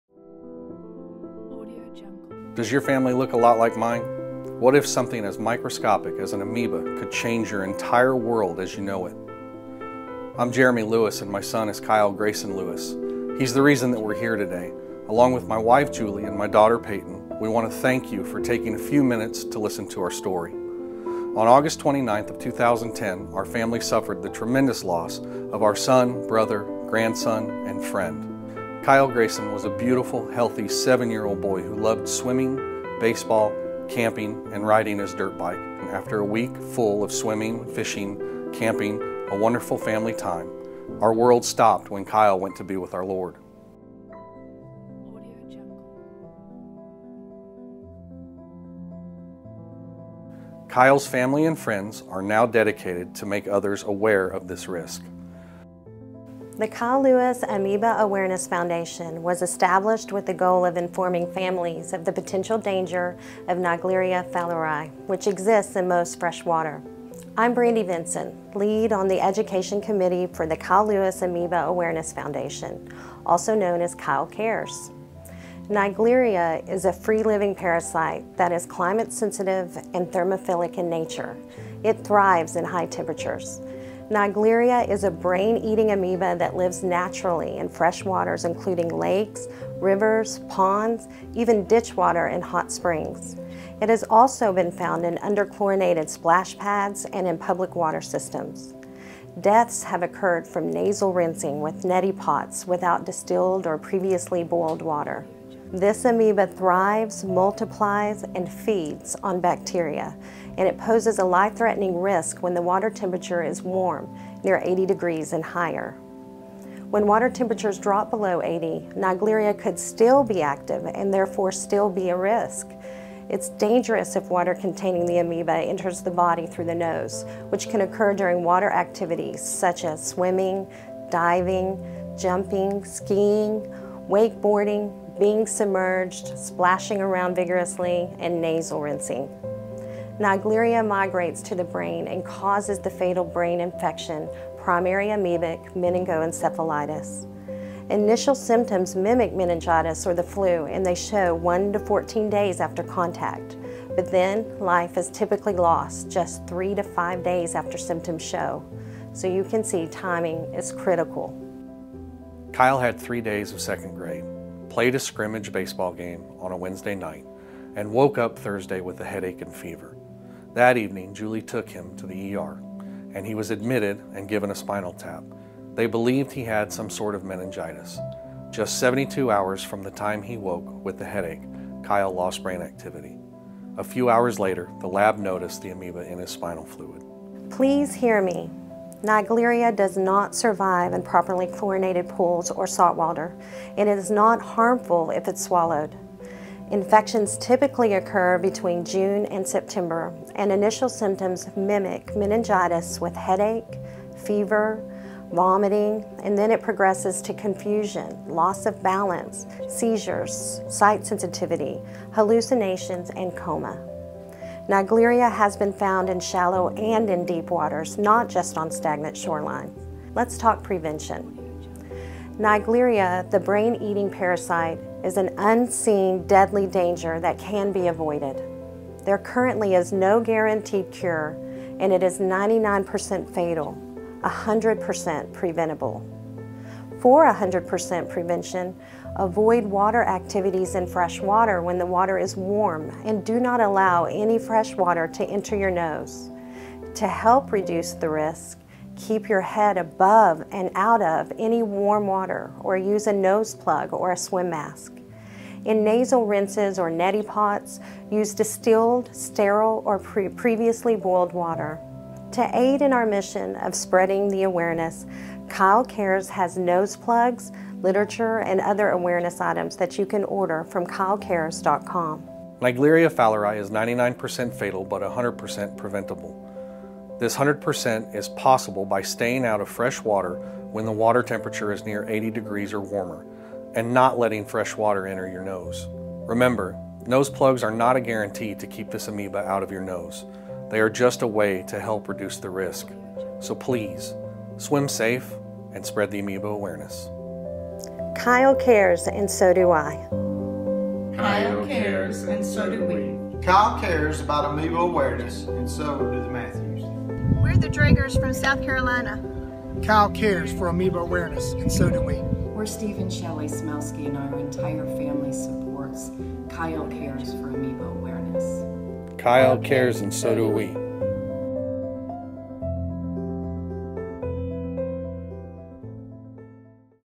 Kyle Cares - 5 minutes for Amoeba Awareness
Naegleria Fowleri, the brain-eating amoeba, is 99% Fatal but 100% Preventable. Please watch and share Kyle Cares' 5 minutes for Amoeba Awareness video and help us arm others with information so they may protect their loved ones.